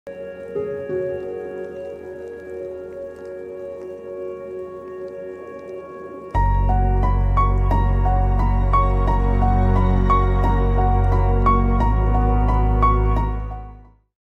Portable Fountain
HoMedics Indoor 3-Tier Relaxation Tabletop Fountain, Automatic Pump with Power Switch, Extra Deep Basin with Natural River Rocks and Reflective Lighting Feature, EnviraScape Silver Springs.